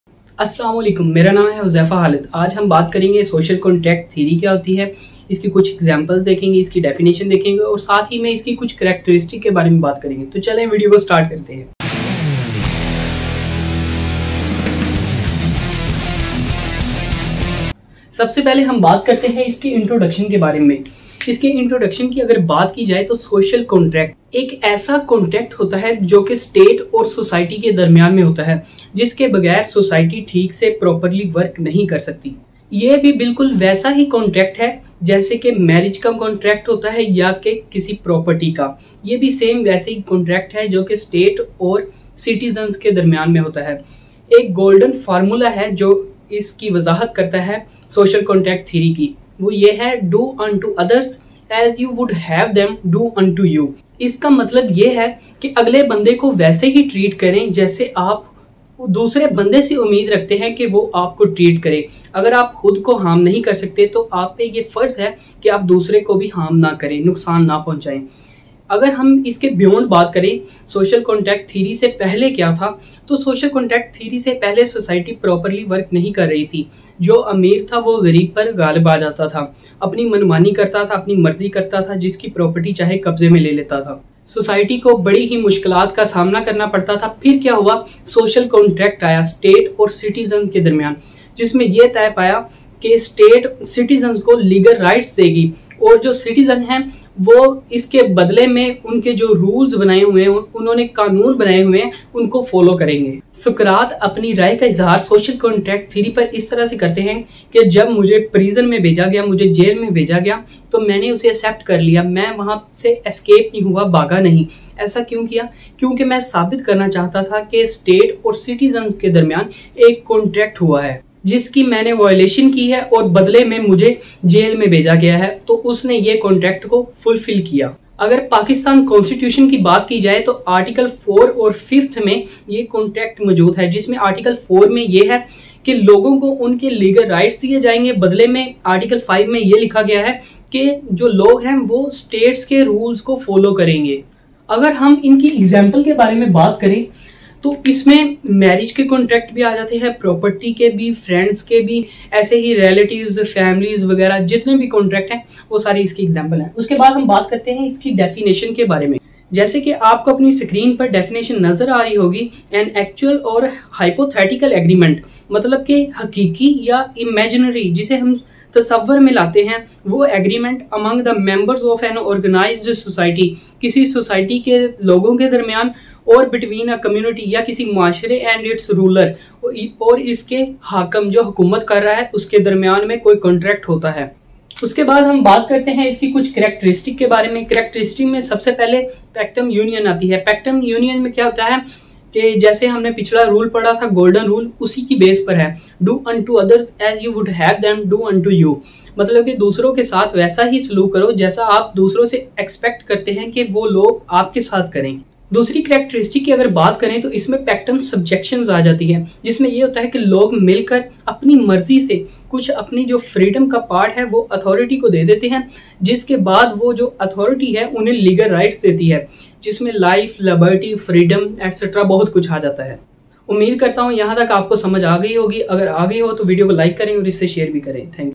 Social Contract Theory | intro | definition | Characteristics in Urdu/Hindi | huzaifa dogar
in this video I discussed the social contract theory in simple and easy ways and this video has 2 parts go on this channel Huzaifa Dogar and search for social contract theory part 2